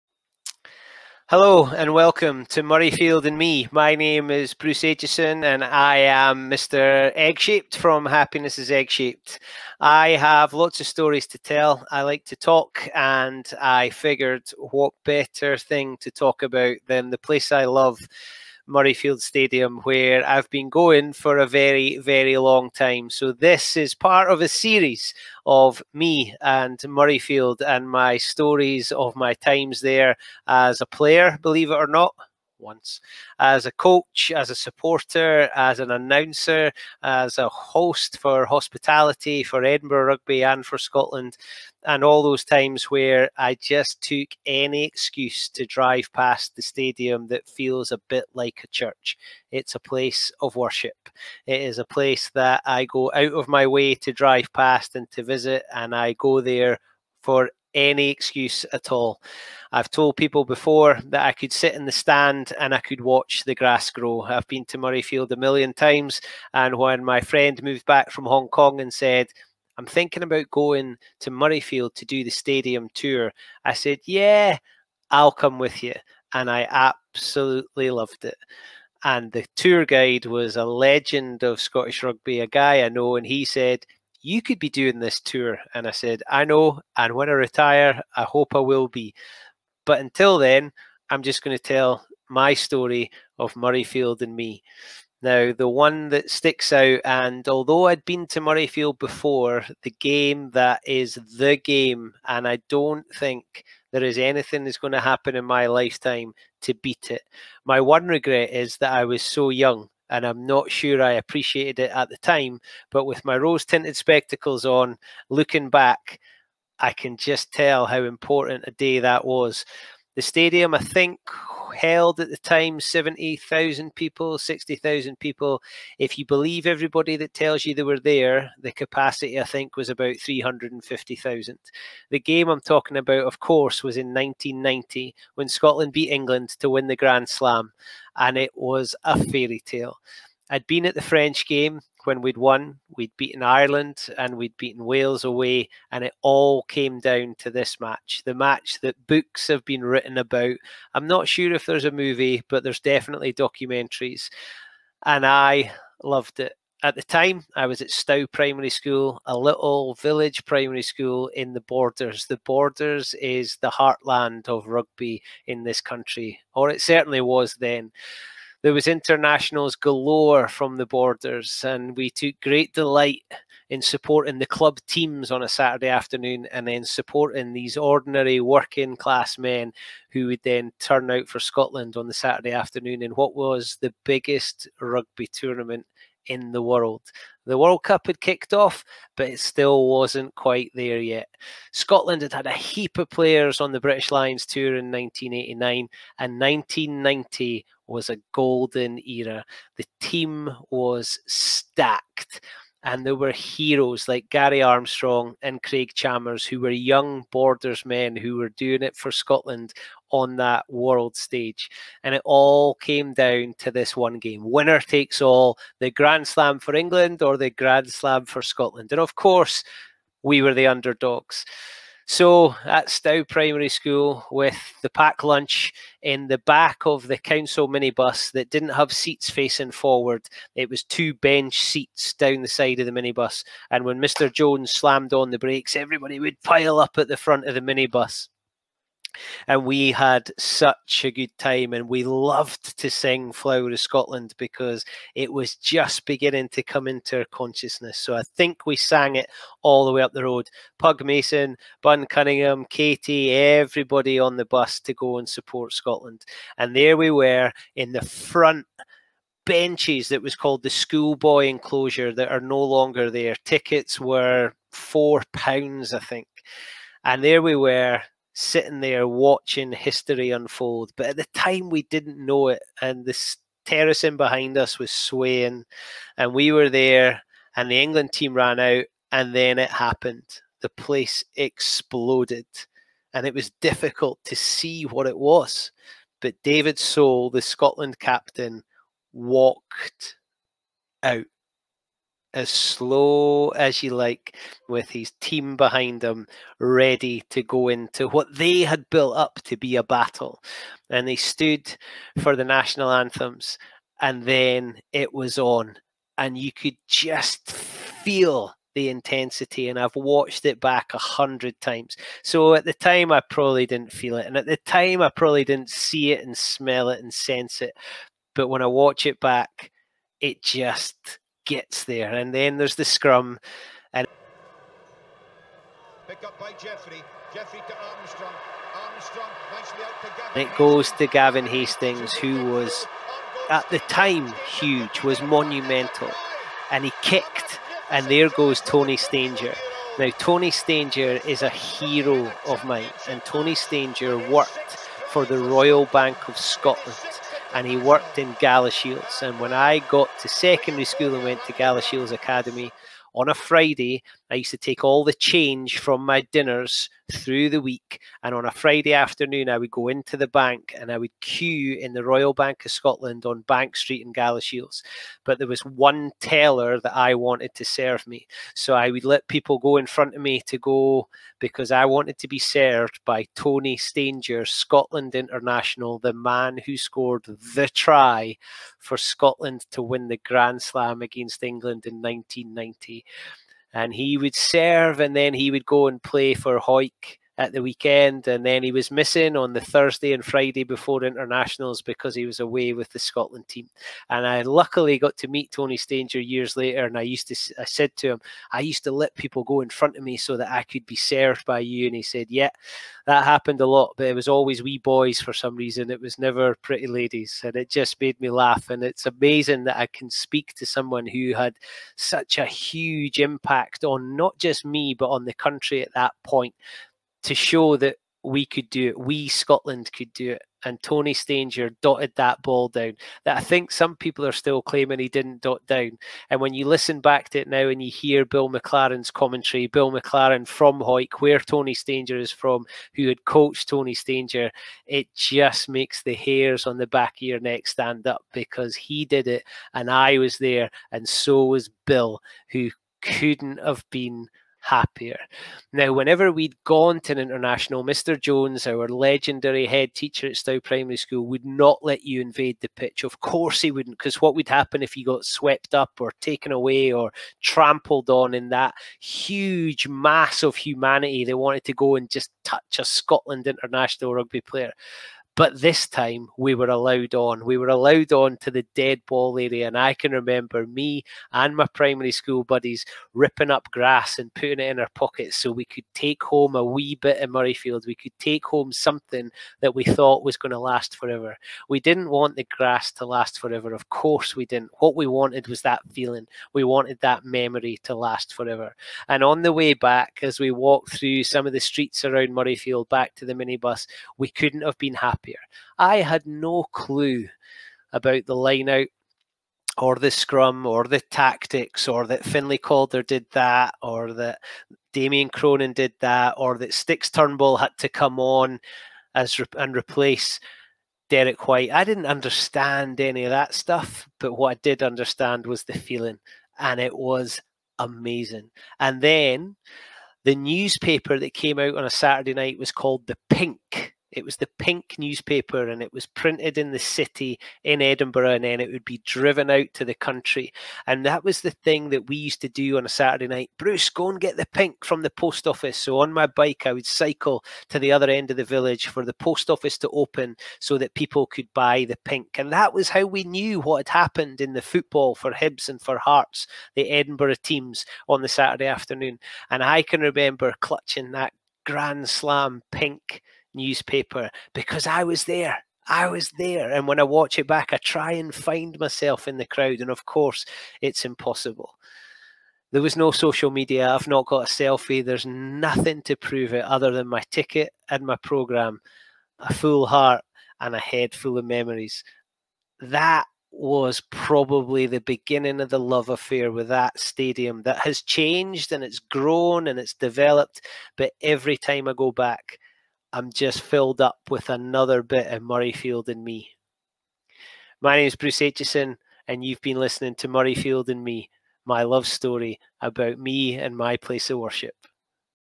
Murrayfield & Me - Scotland v England 1990 [Ep 1]
This is episode 1 of Murrayfield & Me, a love story by Bruce Aitchison.

I'm excited on the way there, love going through the gates and never want to leave. I could watch the grass grow and still dream of scoring the try and kicking the goal in front of packed Murrayfield crowd. This is my Theatre of Dreams, and 1990 was the most special of days in front of a packed Murrayfield crowd.